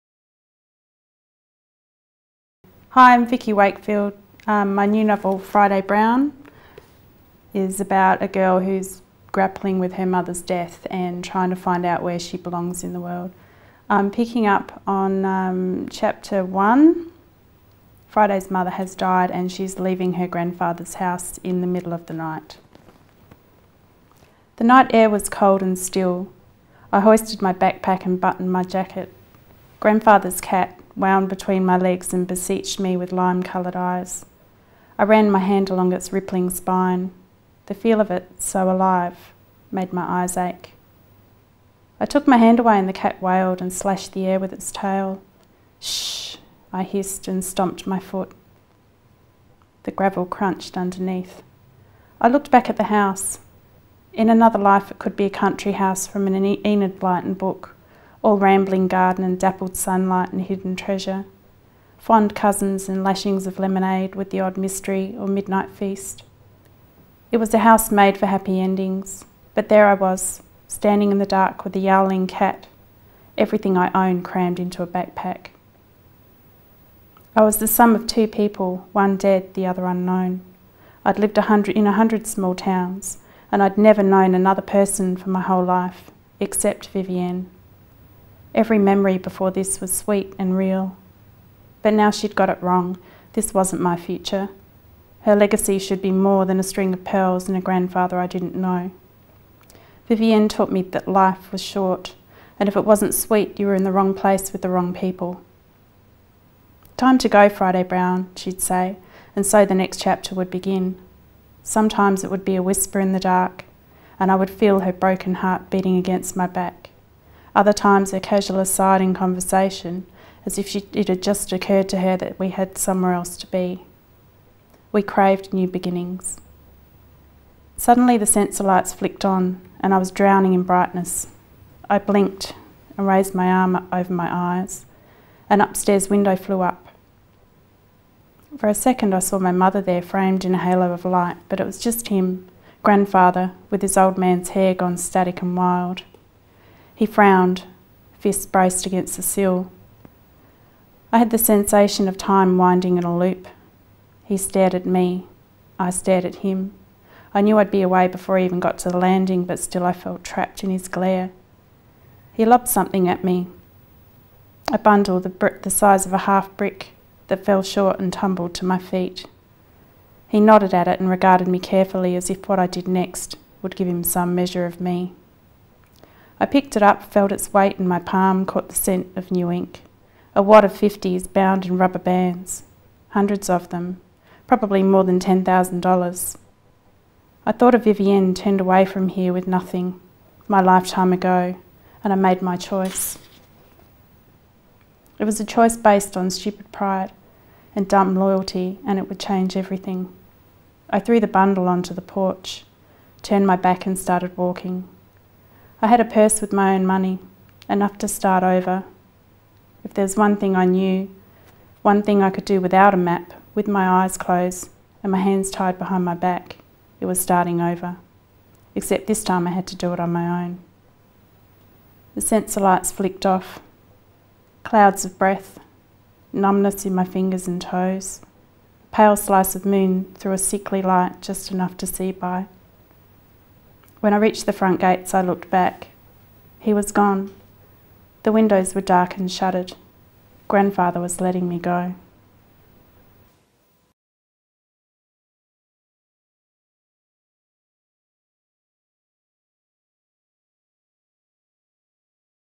Vikki Wakefield on FRIDAY BROWN
FRIDAY BROWN is the breathtaking second novel from the author of the award-winning ALL I EVER WANTED.

Seventeen-year-old Friday Brown is on the run—running to escape memories of her mother and of the family curse. And of a grandfather who'd like her to stay. She's lost, alone and afraid.

Silence, a street kid, finds Friday and she joins him in a gang led by beautiful, charismatic Arden. When Silence is involved in a crime, the gang escapes to a ghost town in the outback. In Murungal Creek, the town of never leaving, Friday must face the ghosts of her past. She will learn that sometimes you have to stay to finish what you started—and often, before you can find out who you are, you have to become someone you were never meant to be.